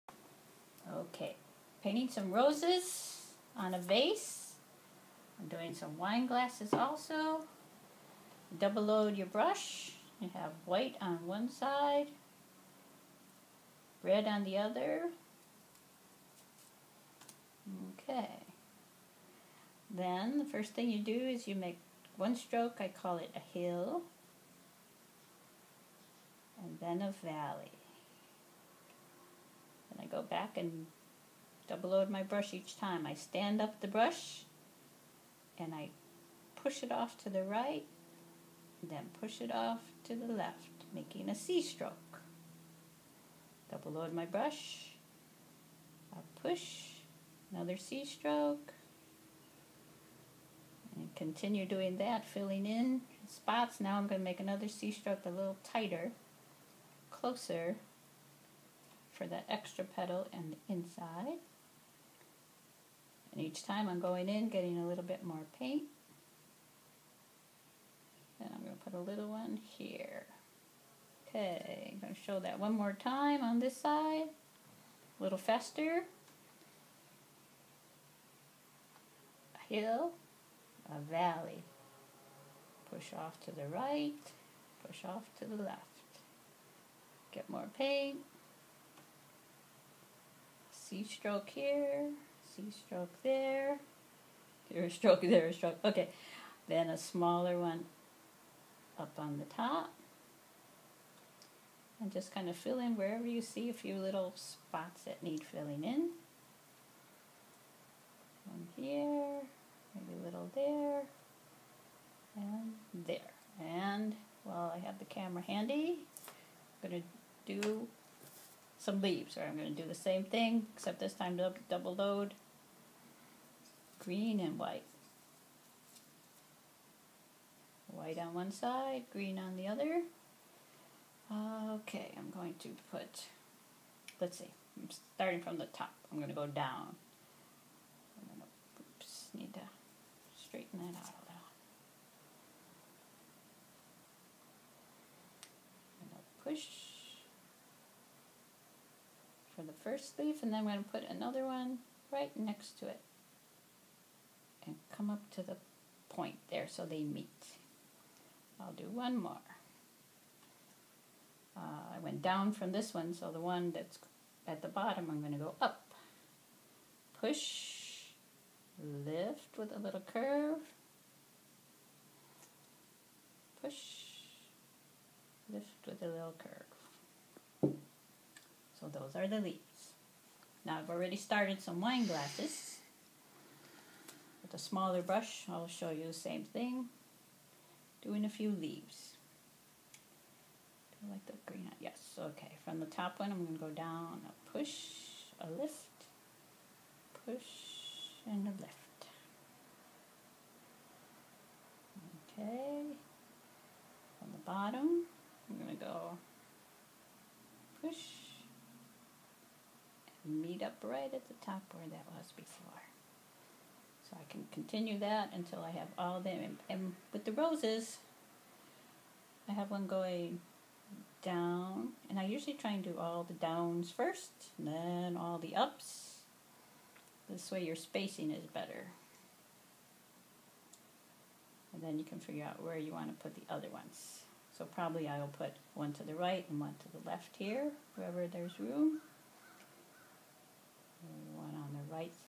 Toni Andrukaitis painting roses on glass
Artist Toni Andrukaitis paints roses on glassware